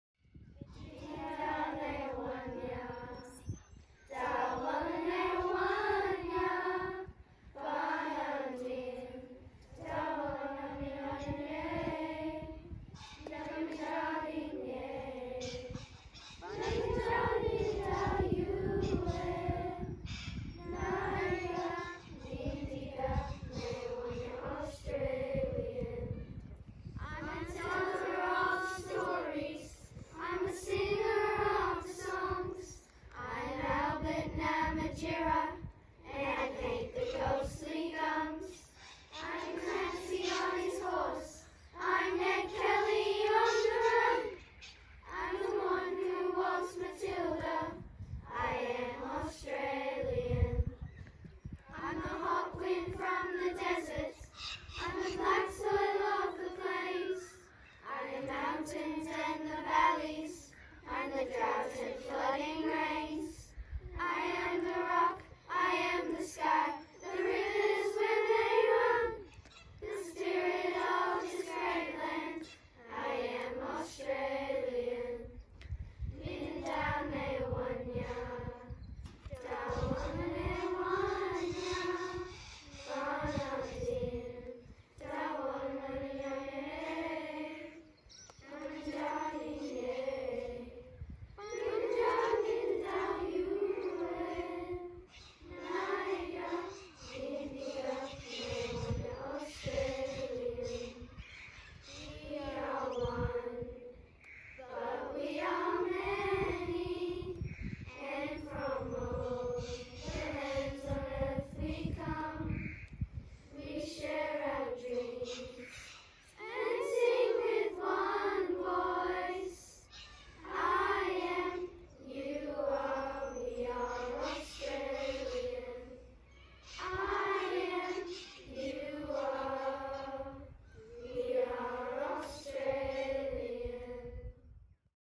NAIDOC Week 2020
National NAIDOC Week 2020 "Always was. Always will be" celebrates the stories from thousands of generations of Australia's First Nations. NAIDOC Week is about encouraging Aussies from all walks of life to work together for a shared future. COVID-19 has put a dampner on 2020 events. Instead here are some of our favourite photos from recent years. Special thanks to the 2019 Year 4 Dhurga choir from St Bernard's Primary School, Batemans Bay for the the music.